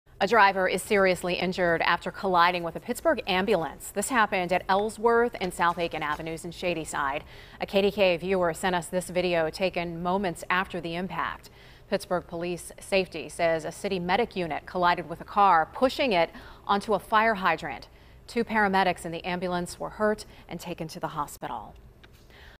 Driver Seriously Injured After Crash Involving Pittsburgh Ambulance
A driver was seriously injured and three paramedics were taken to the hospital after a city ambulance collided with a vehicle Monday afternoon.